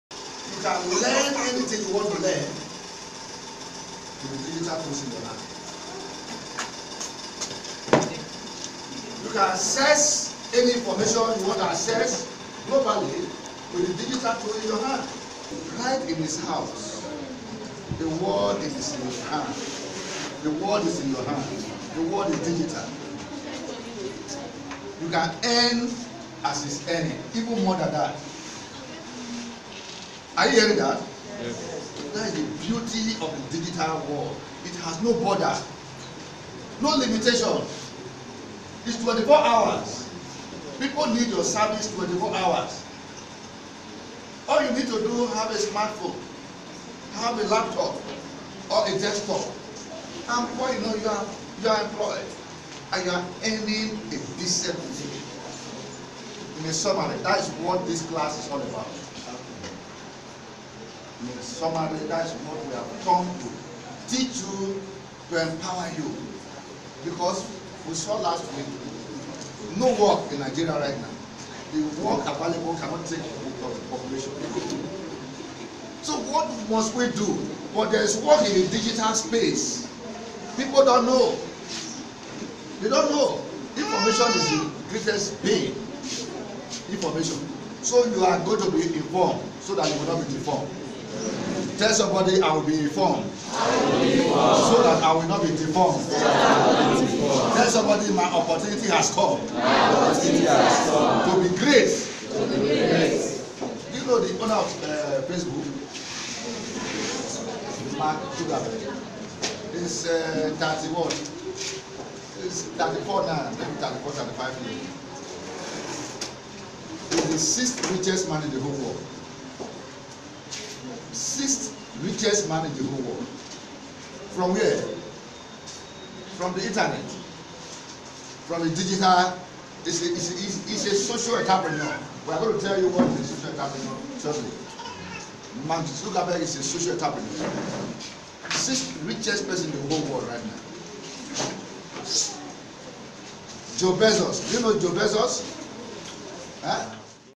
HETAVED LETS GO DIGITAL
LET'S GO DIGITAL IS POWERED BY HETAVED SKILLS ACADEMY AND NETWORKS. IT HOLDS EVERY TUESDAYS @ 10:00A.M. DELTA STATE, NIGERIA.

WHEREBY YOUTHS ARE TRAINED ON HOW TO PROFITABLY MAXIMIZE THE USE OF INTERNET.